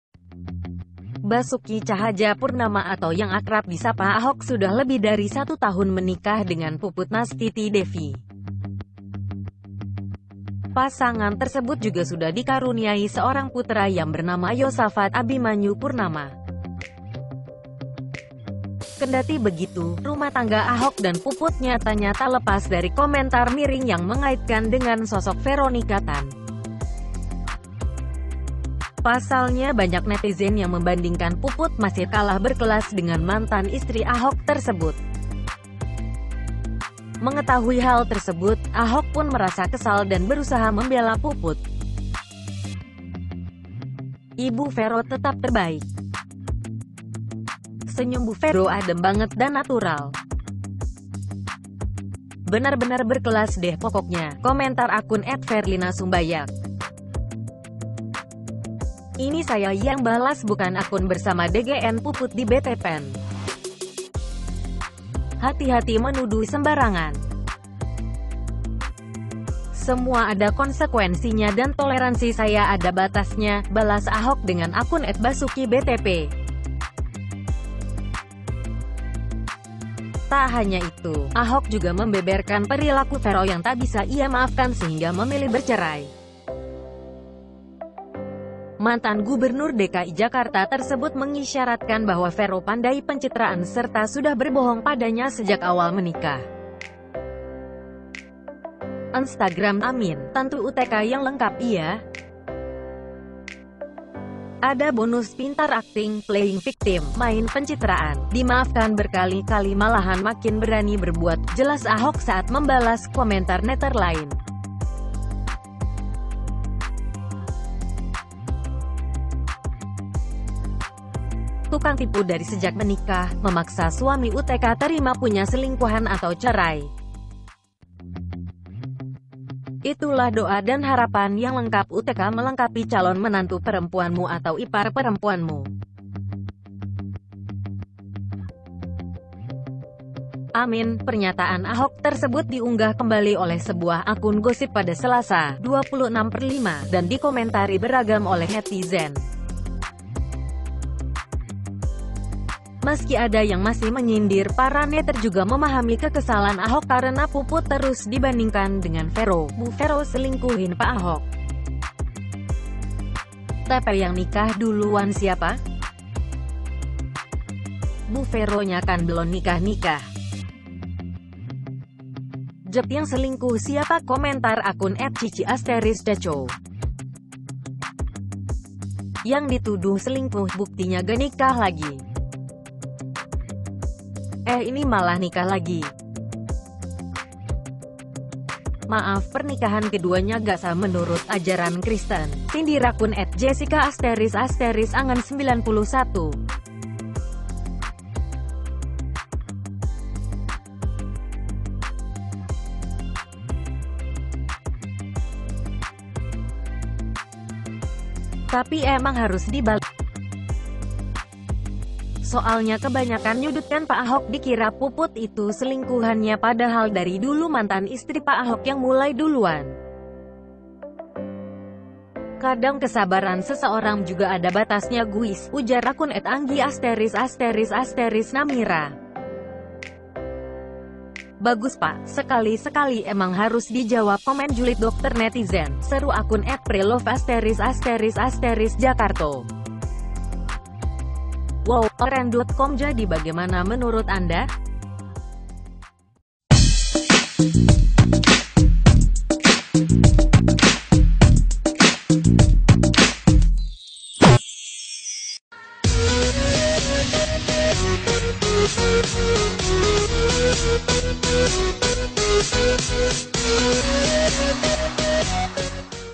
VIRAL HARI INI~Ahok Berusaha Membela Sang Istri Dan Menyebutkan Alasannya Berpisah Dari Vero
REDDOT2 CHANNEL
Mensajikan Konten BERITA DAN INFORMASI Dari Berbagai website Resmi,
terimakasih